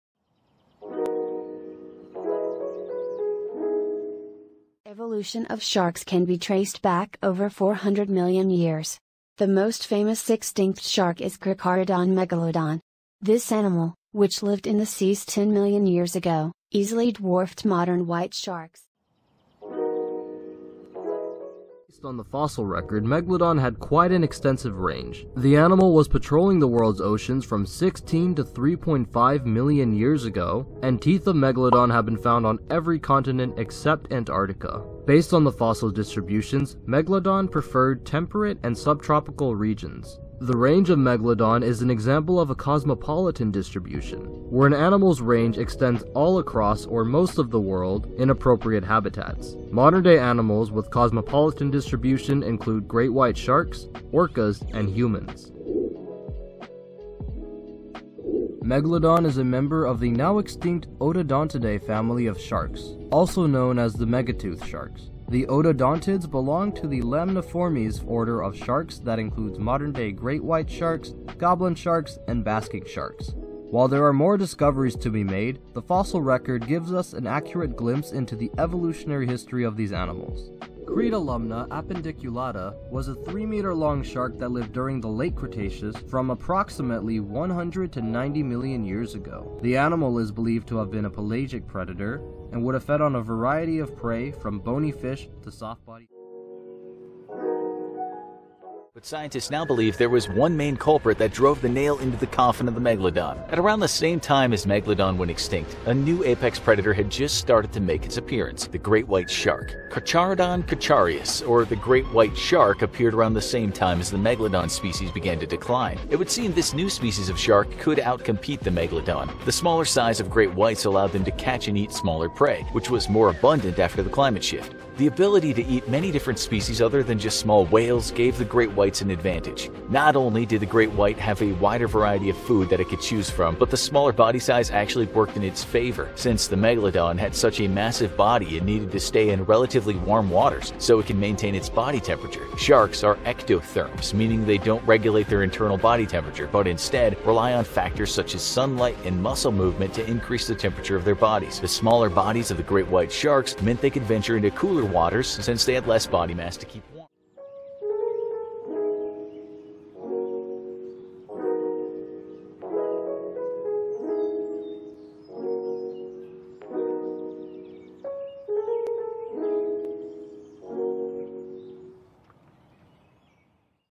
Carcharodon Megalodon at The American museum of Natural History
To know more about the Carcharodon Megalodon at The American museum of Natural History

Visit the American Natural History Museum and see "Carcharodon Megalodon".